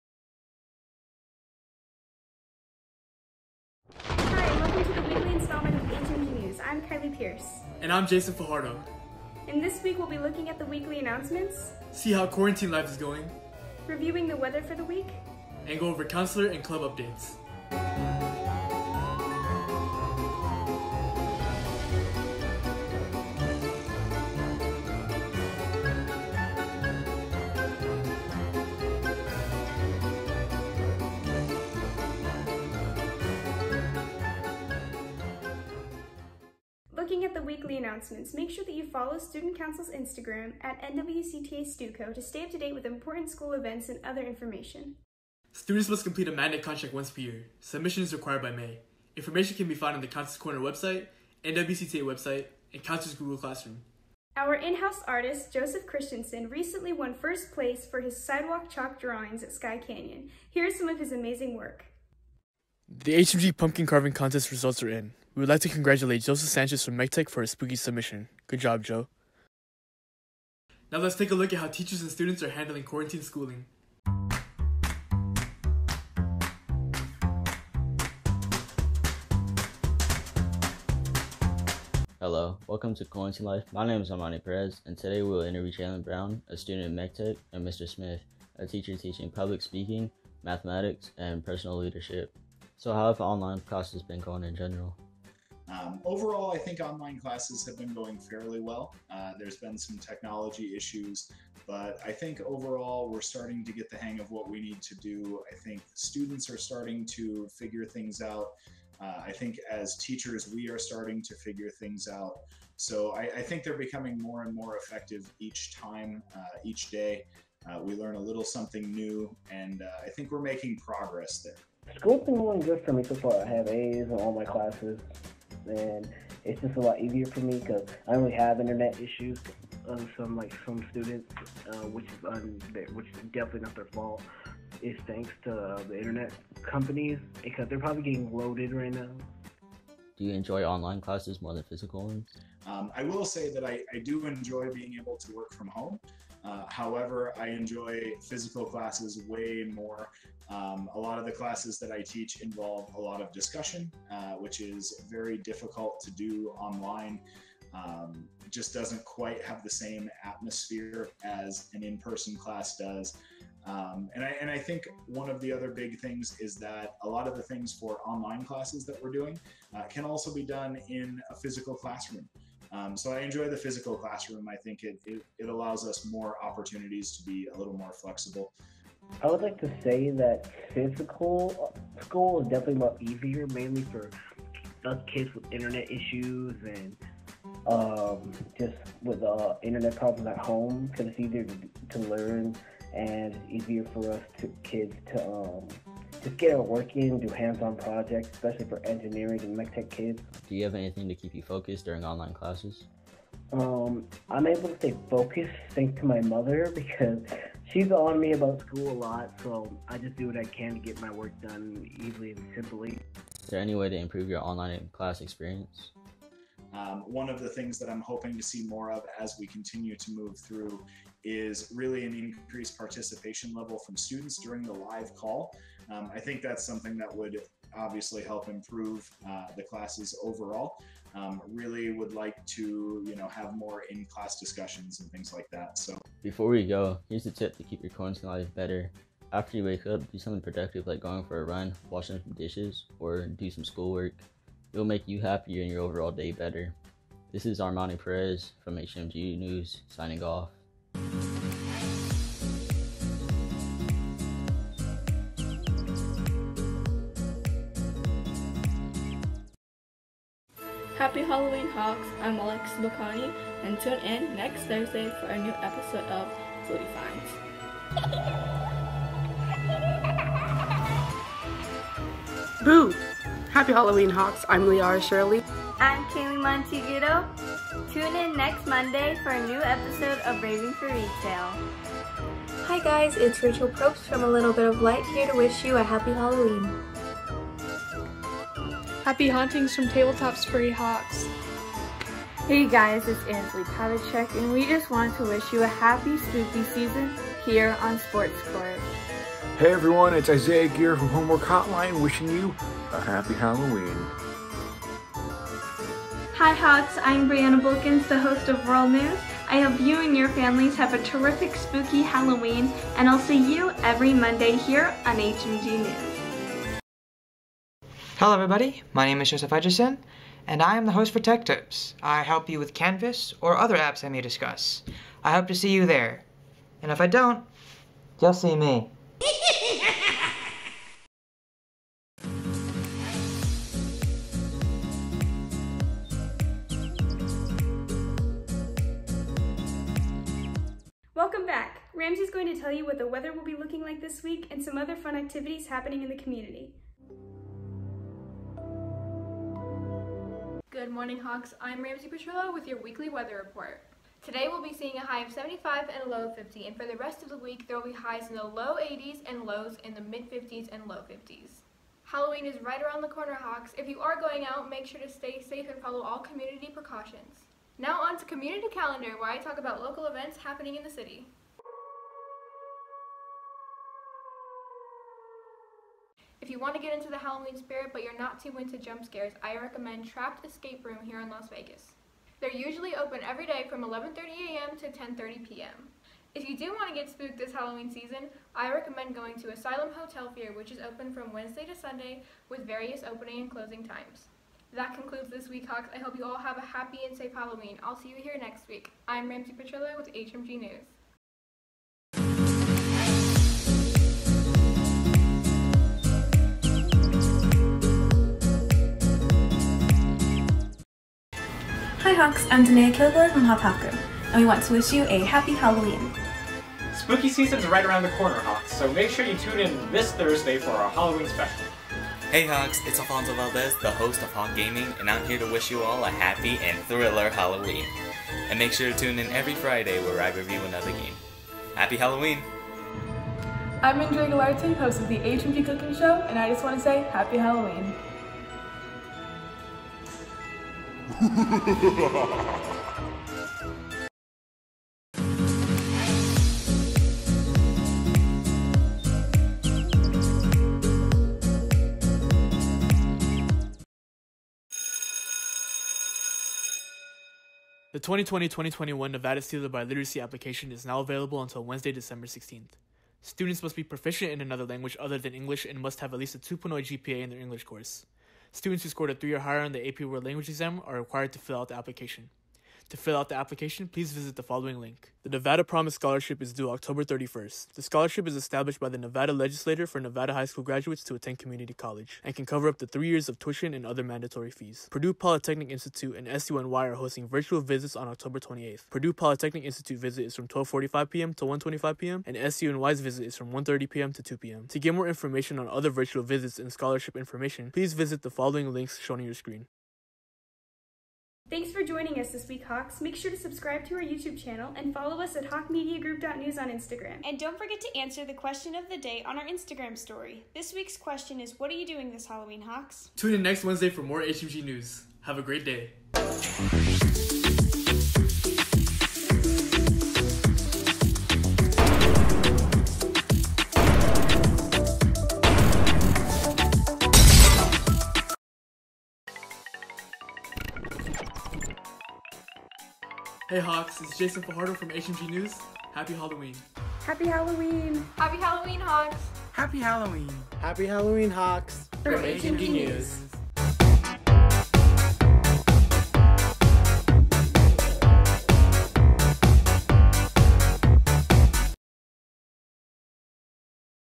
HMG News 10/28/20
Tune in Every Wednesday Morning for our LIVE Newscast with your anchors Kylee and Jason & Weather with Ramsey. This week we featured Armani with Quarantine Life. Have a Great Week and a Happy Halloween!